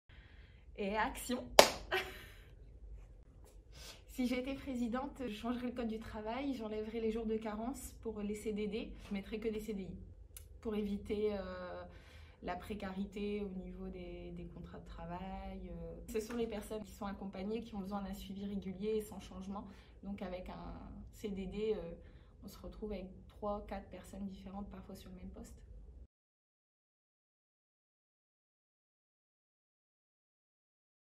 Si j'étais présidente Proposition 11 Louise
"Si j'étais Présidente"
Le délai de carence entre deux contrats génère à la fois de la précarité et une perte de chance pour les personnes accompagnées car les professionnels qui les suivent se succèdent sans un véritable suivi.
Merci Louise pour votre proposition!